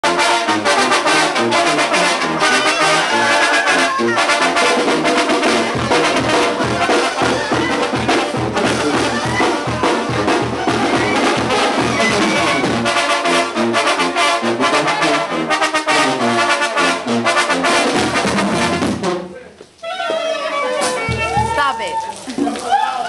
BANDA DE MUSICA EN QUITUPAN
LA BANDA TOCANDO EN LA  PRESIDENCIA DE QUITUPAN